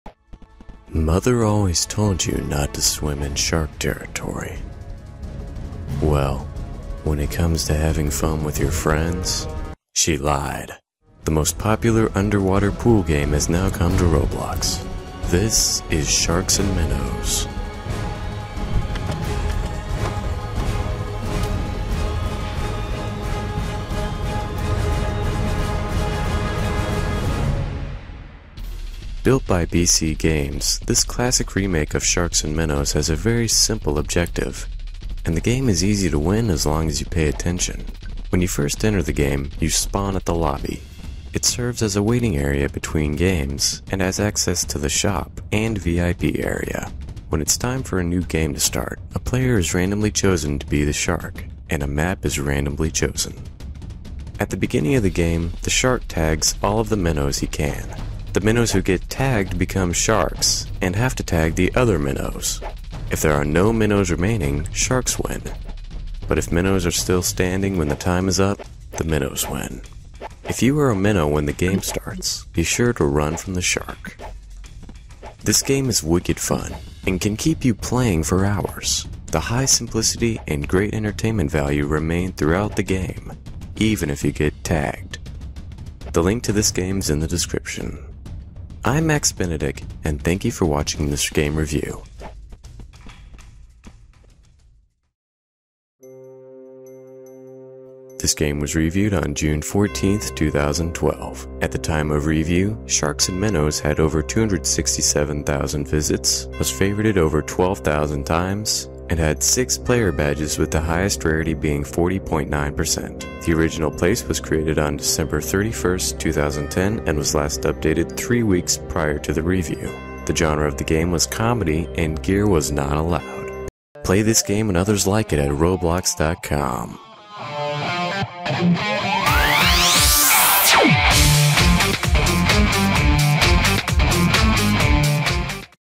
Game Review - Sharks and Minnows
Creator: BCGames

This game is a classic Robloxian remake of the popular Sharks and Minnows pool game. Run from the sharks and try not get tagged. But if you're the shark, be sure to tag all the minnows you can!